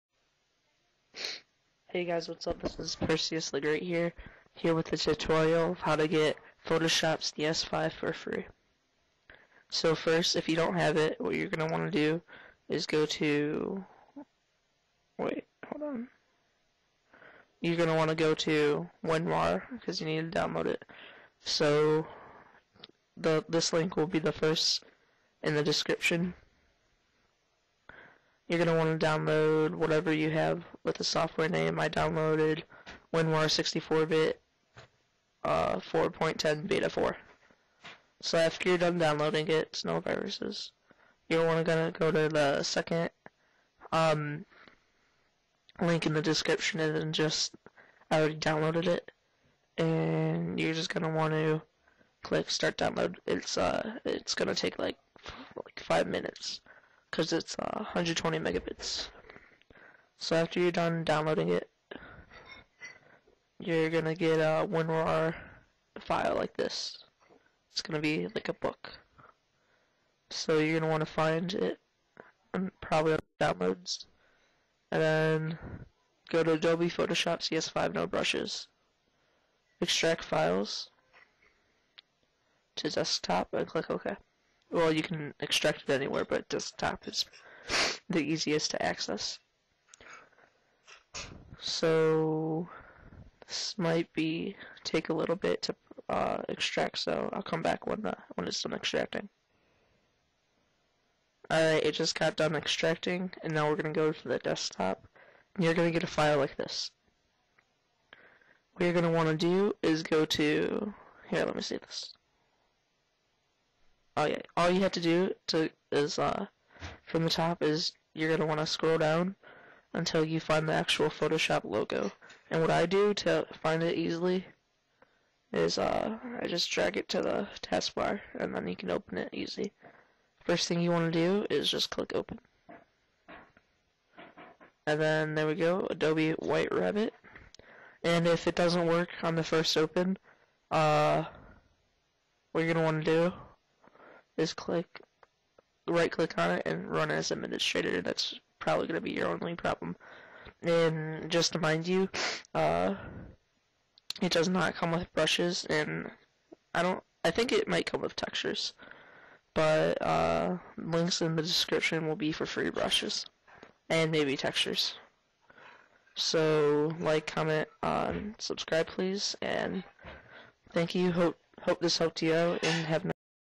How to get PhotoShop CS5 for free (no brushes)
This is a tutorial on how to get photoshop cs5 for free.

Free Brushes! :D